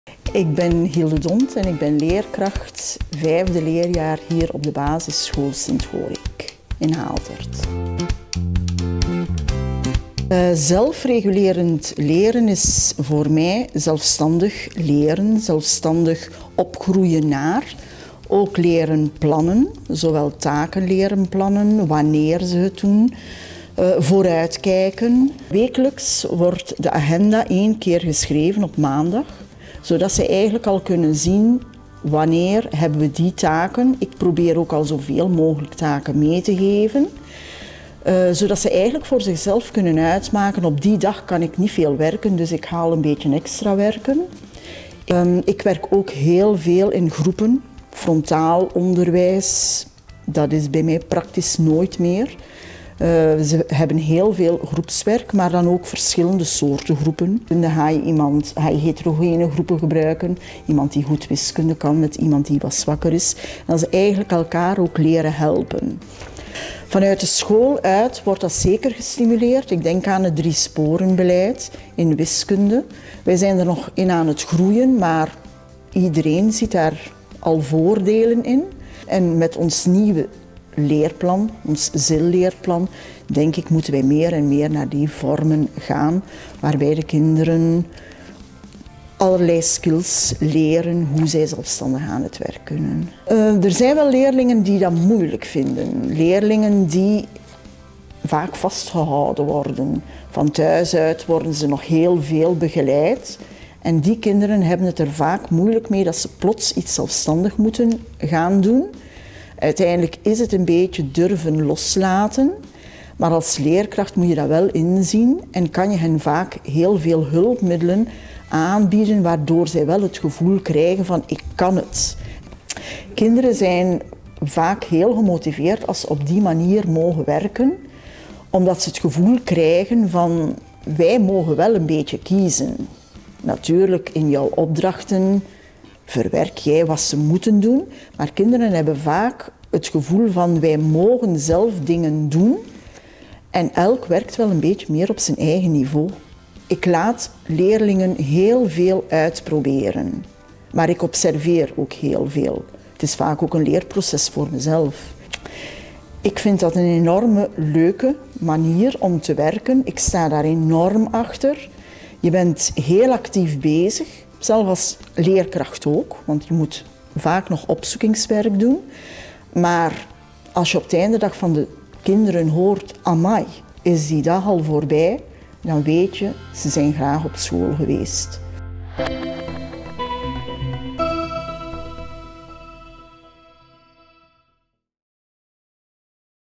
Zelfregulerend leren: zo pak je dat aan in je klas
Juf Hilde Dhondt heeft het over de werking en voordelen van zelfregulerend leren in je klas.
Nog meer te weten komen over dit thema? Surf naar